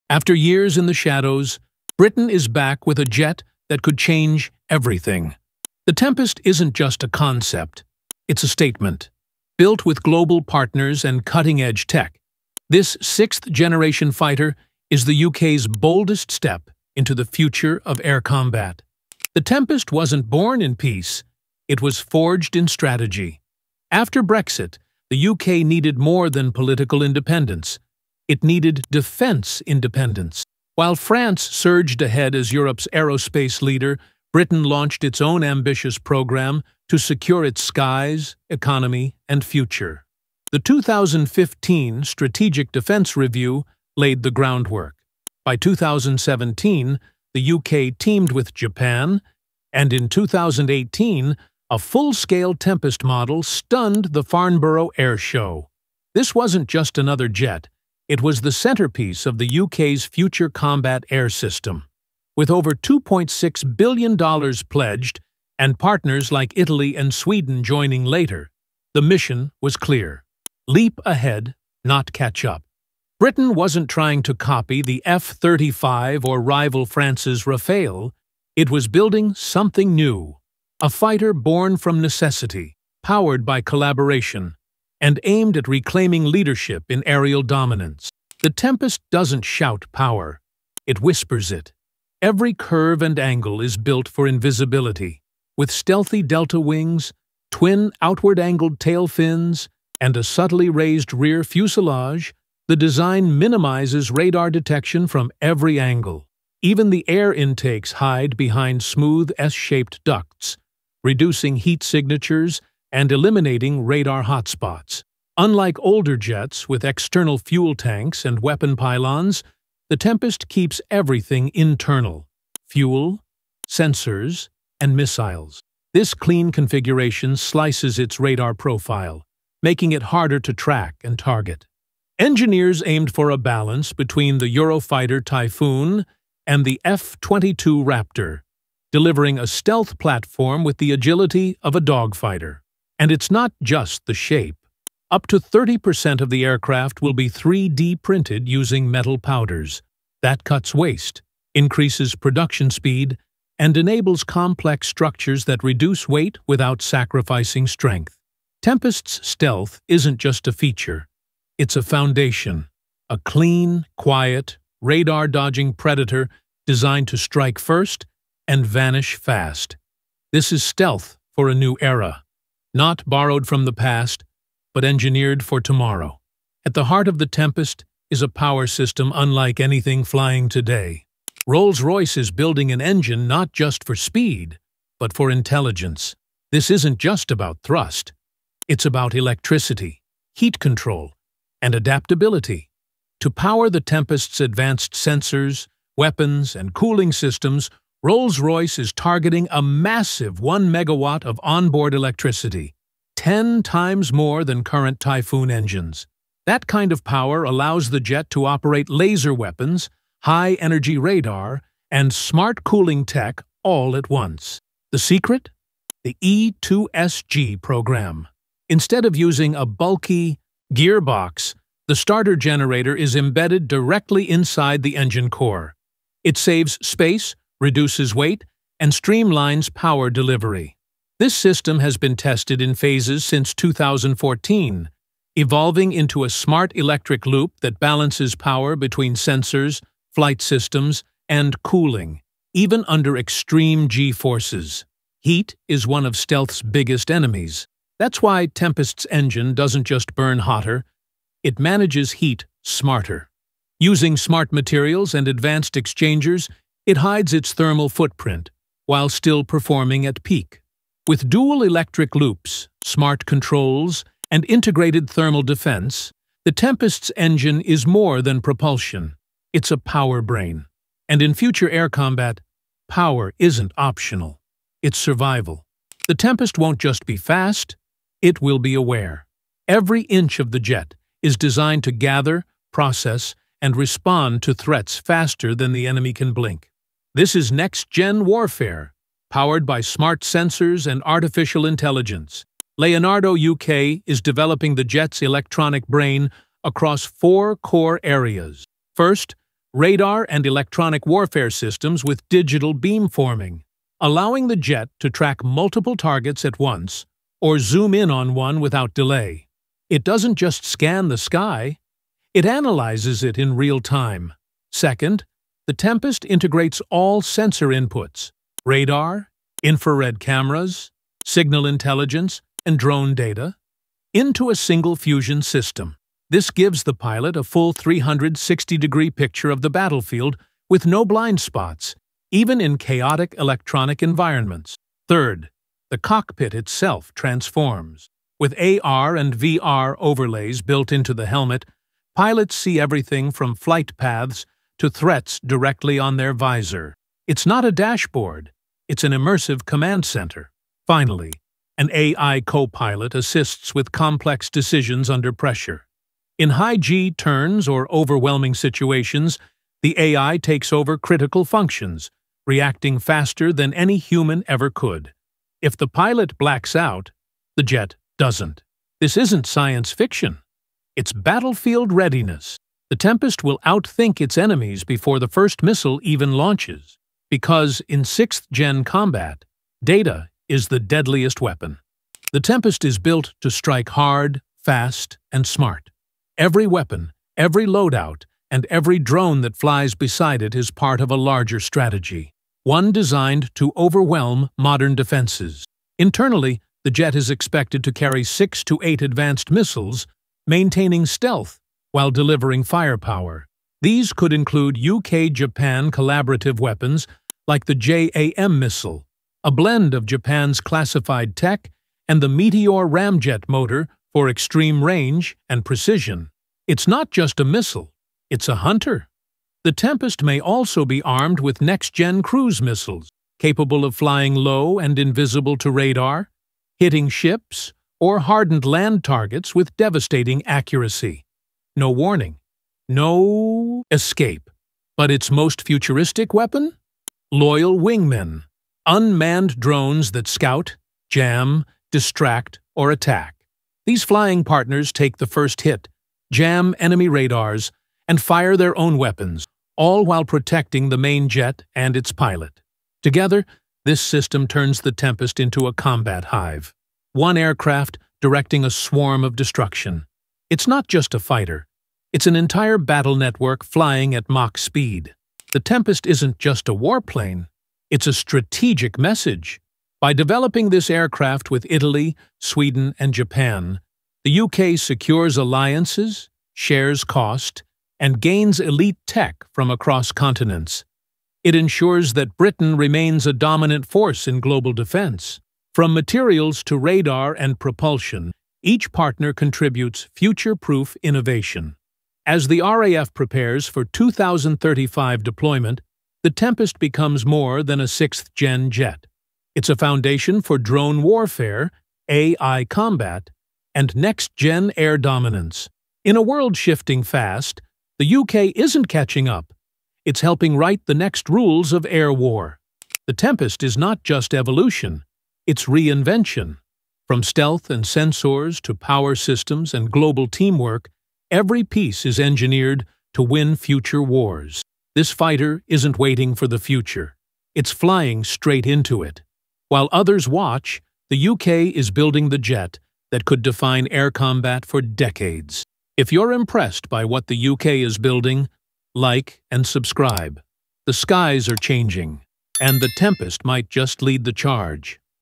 UK's BIG Mistake Replacing Typhoon with Tempest 6th Gen Fighter Jet
Finally: UK Unveils Tempest 6th-Gen Fighter Jet to Replace Typhoon!

Aviation Heights brings you the most advanced breakthroughs in global air combat. In this video, we break down the UK’s ambitious sixth-generation Tempest fighter jet, revealing how it’s set to replace the aging Eurofighter Typhoon. From stealthy designs and AI co-pilots to advanced engine systems and swarm drone tactics — we cover everything that makes the Tempest one of the most futuristic aircraft ever developed.

🔧 This isn’t just a jet — it’s a strategic shift in aerial warfare.
🎯 Explore the design, power, weapons, and global alliances behind this multi-billion-dollar program.